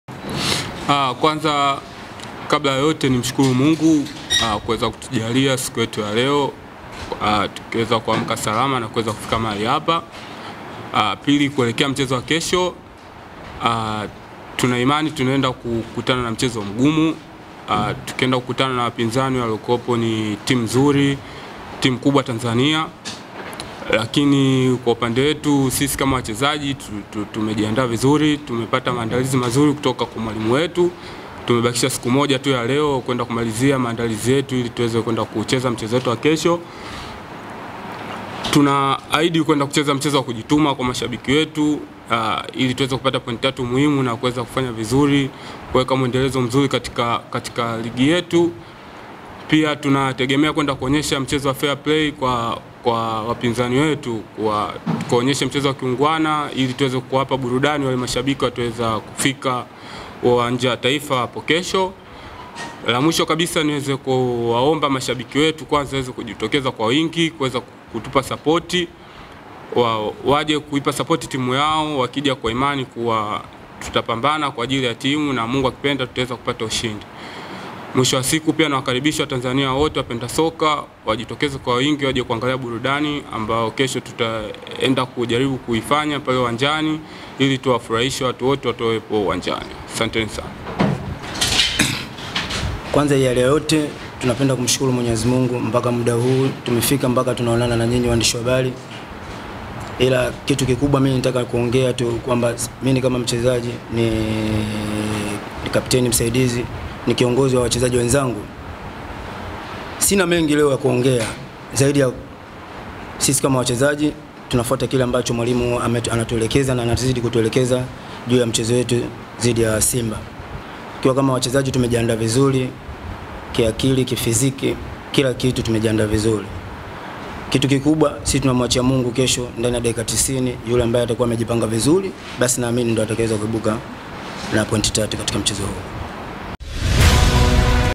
YANGA vs SIMBA: Vita ya Wabelgiji Ilivyowafanya Manahodha Wazungumze 'Kinyoonge'
Manahodha wa timu za Yanga na Simba, Juma Abdul na John Bocco leo walionekana kuzungumza bila bashasha yoyote kuelekea mchezo wa kesho utakaozikutanisha timu zao. Hii ni tofauti na mchezo uliopita ambap waliongea kwa kujiamini zaidi.
Hii ni vita ya makocha Wabelgiji wawili, Luc Eymael na Sven Vandenbroeck